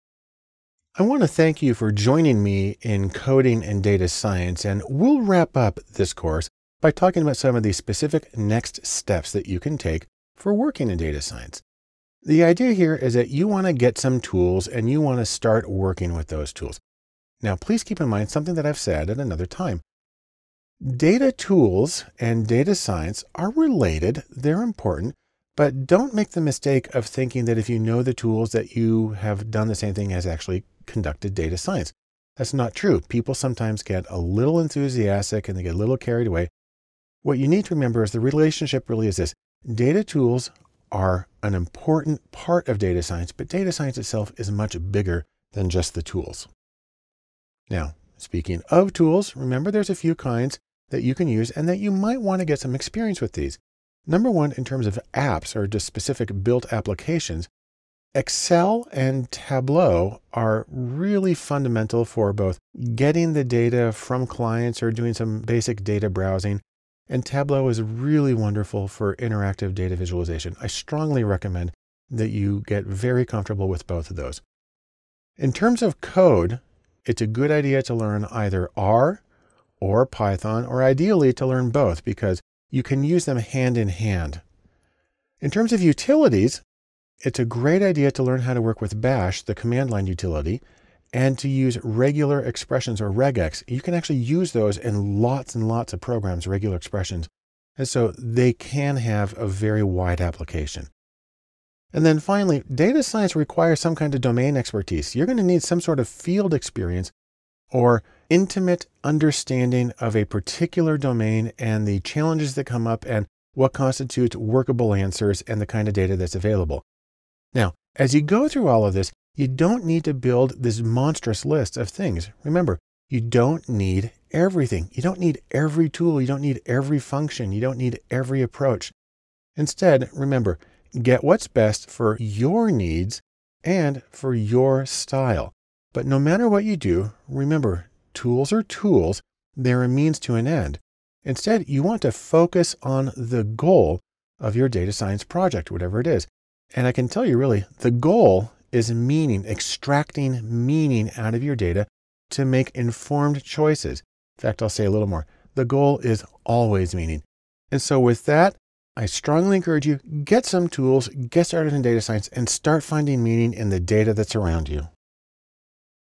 Next Steps - Coding - 6.1
Coding is an important part of data science but it remains only ONE of the important elements. Find out what else you can do to get started in data science. For more on this topic – and all of data science!